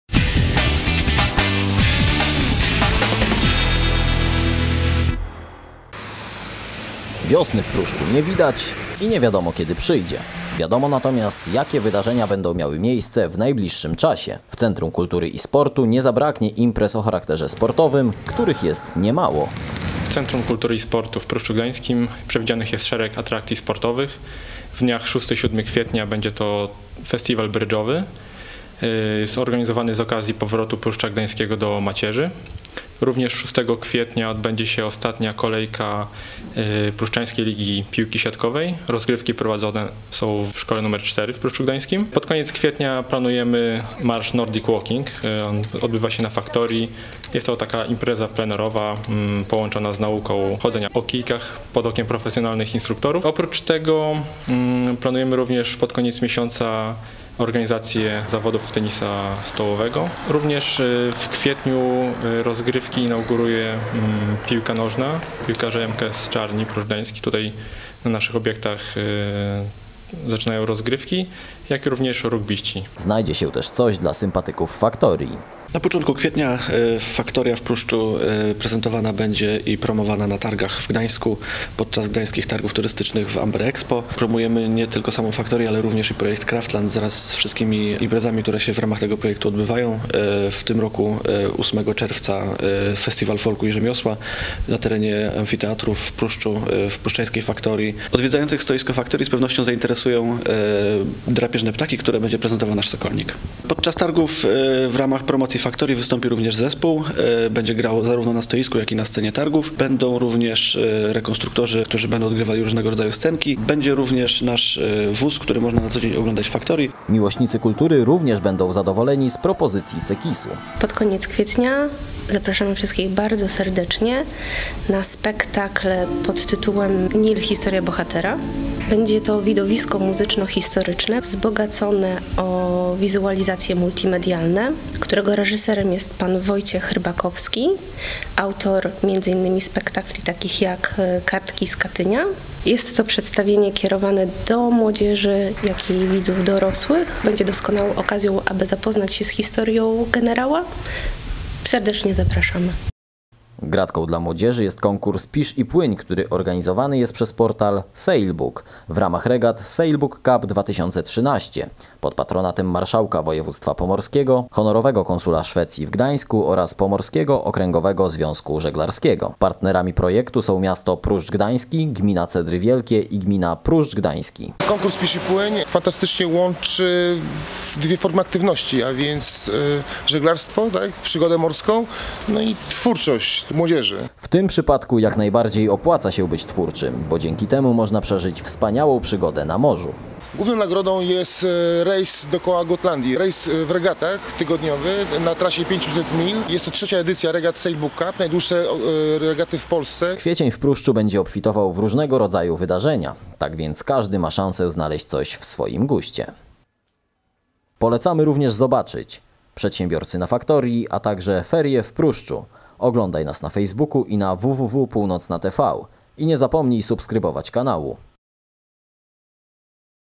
Co w Pruszczu piszczy
Wiosny w Pruszczu nie widać i nie wiadomo kiedy przyjdzie, wiadomo natomiast jakie wydarzenia będą miały miejsce w najbliższym czasie. W Centrum Kultury i Sportu nie zabraknie imprez o charakterze sportowym, których jest nie mało.